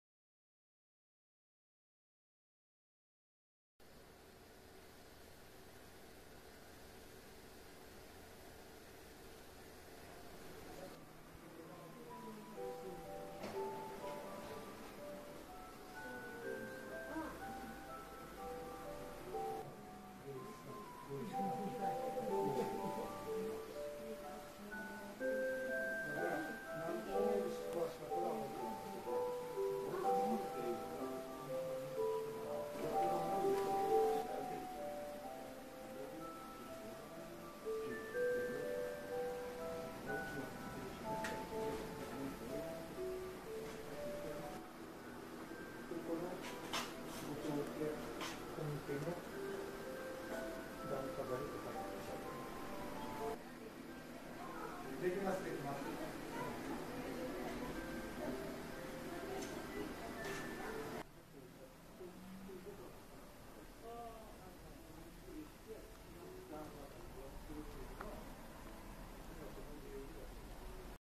豪商屋敷彩るひな飾り５００点
大正期のひな人形や薩摩藩由来の金助まりなど約５００点を飾る「雛〔ひな〕の宿」が、都城市高城の国有形文化財「旧後藤家商家交流資料館」で開かれている。